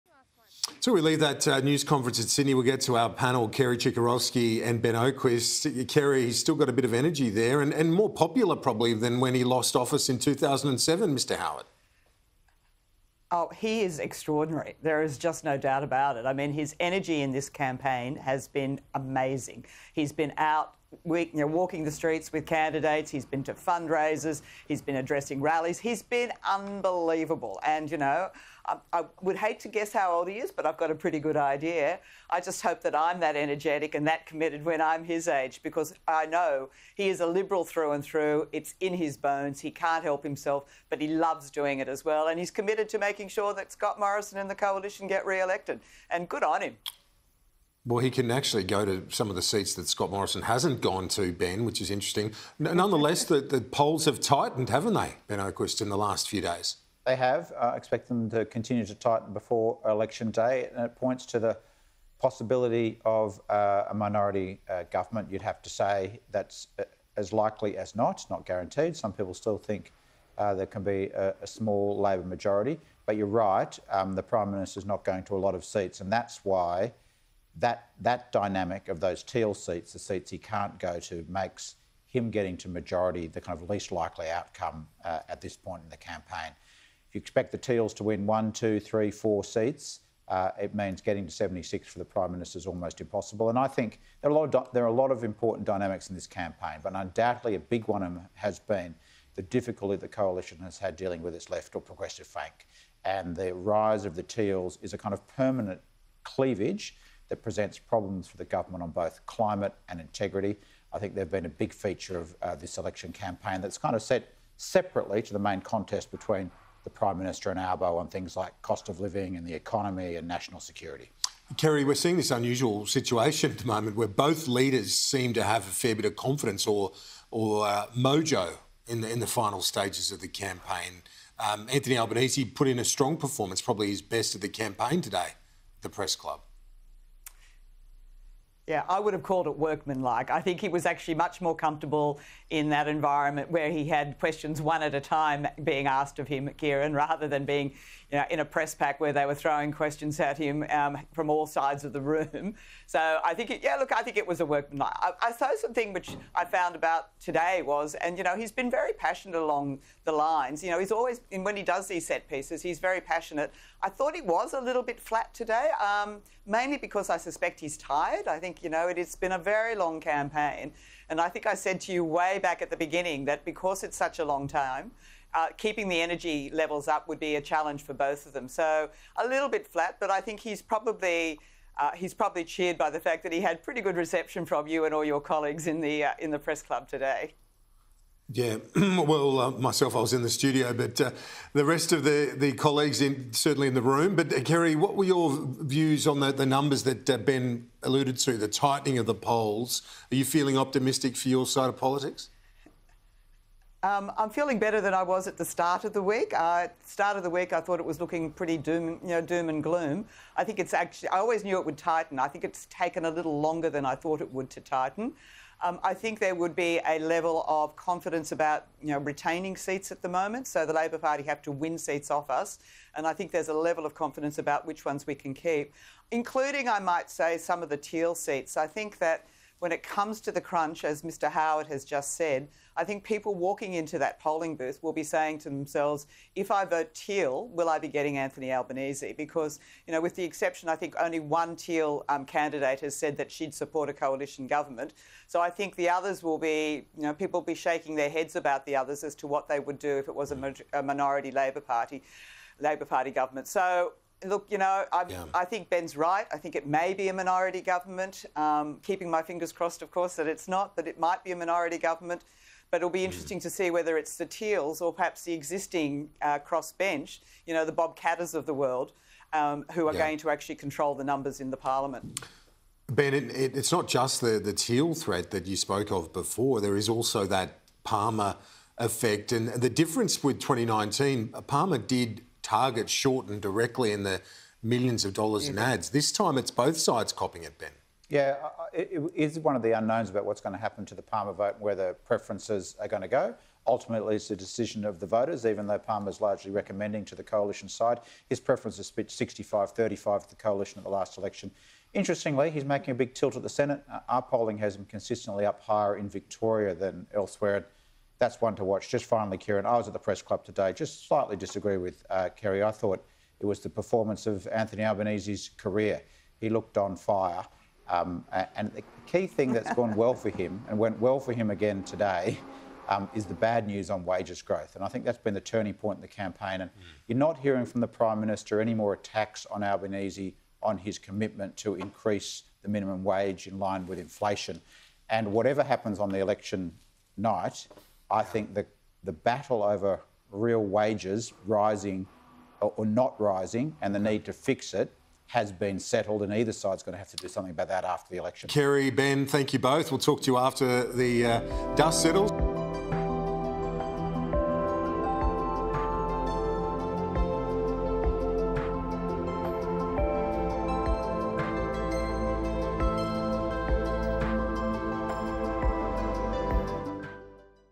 Final Stretch of the Election Campaign | Ben Oquist on Sky News
"The rise of the teals is a kind of permanent cleavage that presents problems for the government on both climate and integrity. I think they've been a big feature of this election campaign, that's kind of set separately to the main contest between the Prime Minister and Albanese on things like cost of living and the economy and national security."

- Ben Oquist, Executive Director at the Australia Institute

Originally aired 18th May 2022.